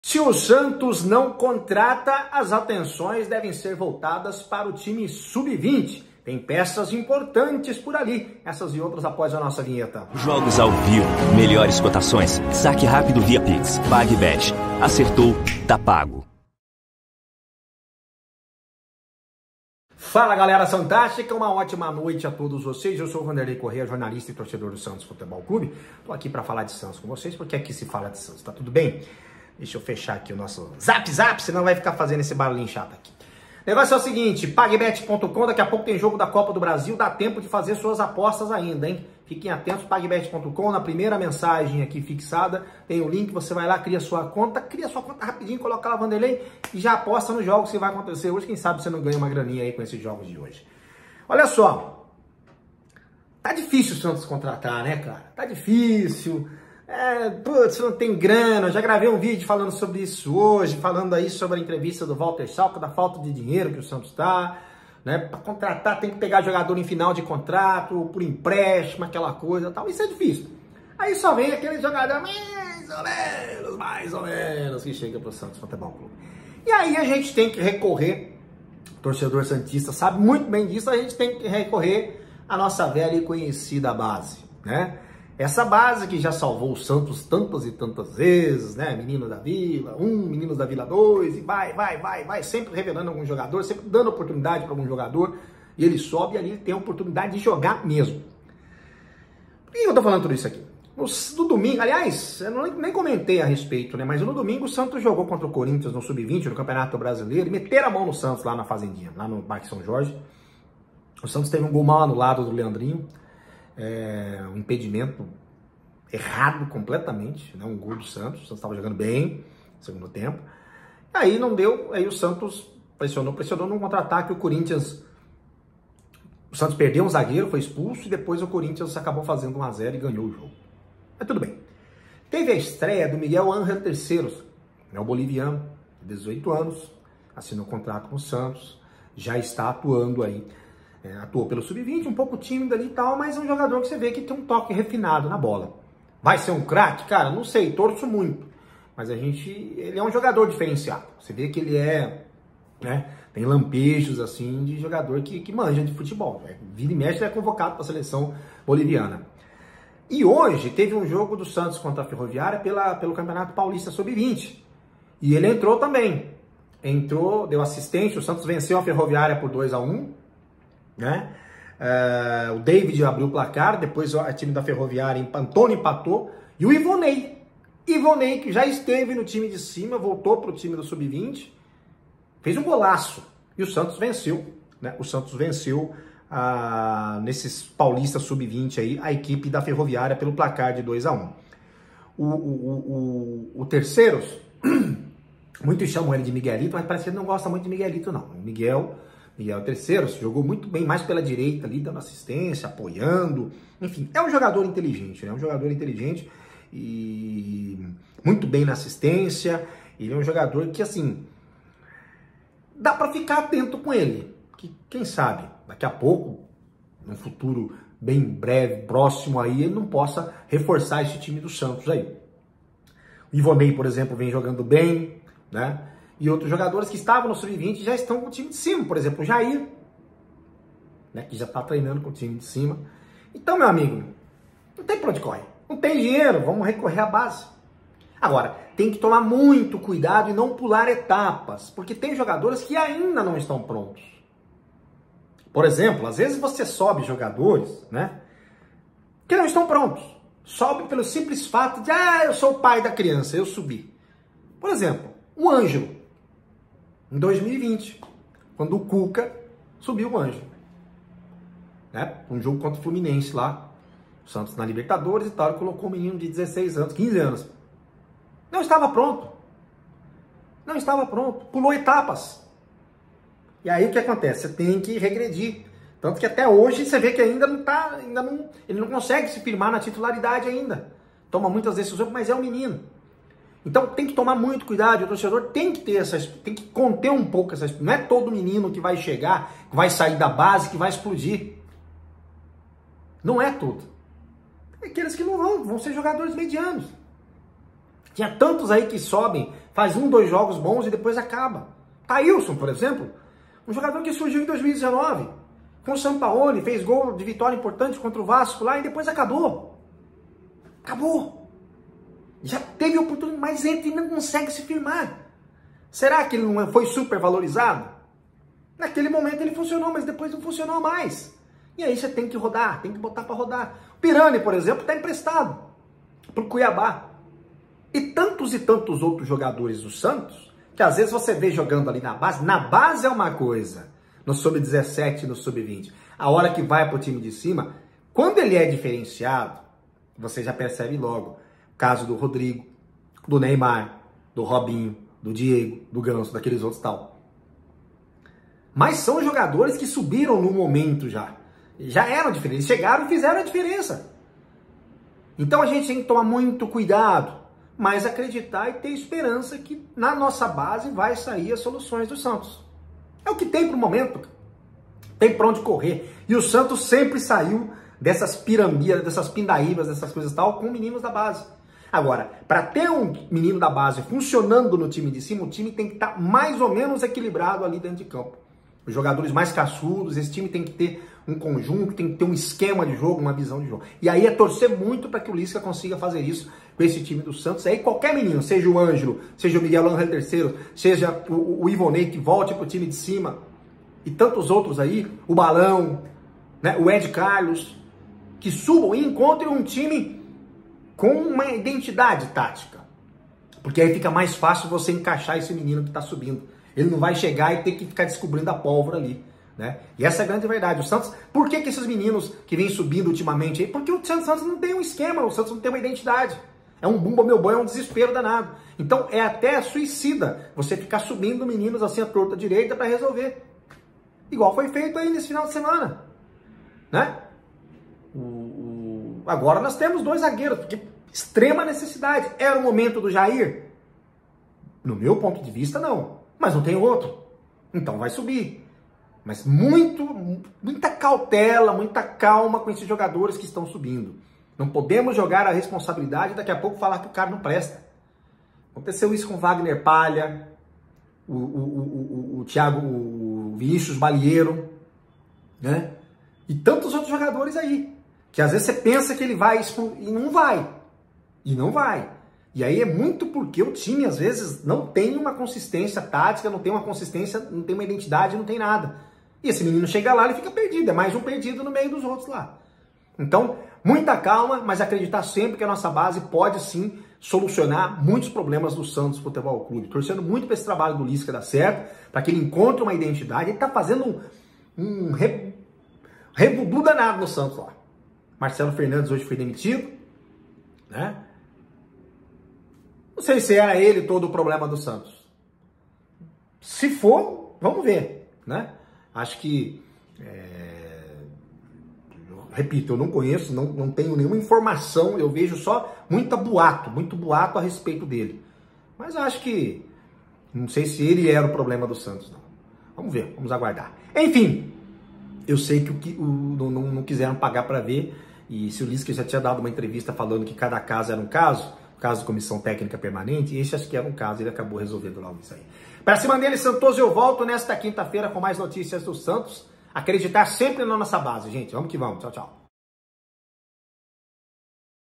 MIguel Terceros se destaca na vitória do Santos no Paulista sub 20
O Santos venceu a Ferroviária hoje no CT Rei Pelé por 2 a 1 e MIguel Terceros foi um dos destaques da partida. Além de atuar muito bem pela direita, ele deu assistência para o gol de Ivonei, que aliás foi o gol da vitória. Muita calma com os nossos meninos.

PAGBET
ACERTOU. TÁ PAGO.
CRIE A SUA CONTA NO PAGBET.COM
"LINK PAGBET NO COMENTÁRIO FIXADO"